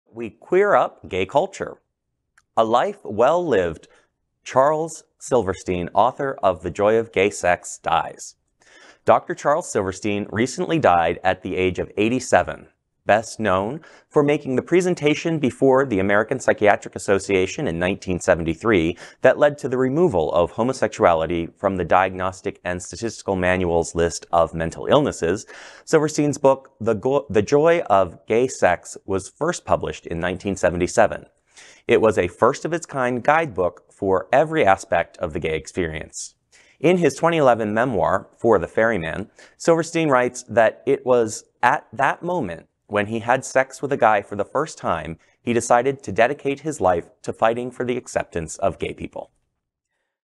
A Life Well Lived' Charles Silverstein, Author Of 'The Joy Of Gay Sex' Dies
Dr. Charles Silverstein recently died at the age of 87. Best known for making the presentation before the American Psychiatric Association in 1973 that led to the removal of homosexuality from the Diagnostic and Statistical Manual’s list of mental illnesses, Silverstein’s book The Joy of Gay Sex was first published in 1977. It was a first-of-its-kind guidebook for every aspect of the gay experience.  In his 2011 memoir, For the Ferryman, Silverstein writes that it was at that moment when he had sex with a guy for the first time, he decided to dedicate his life to fighting for the acceptance of gay people.

Gay Culture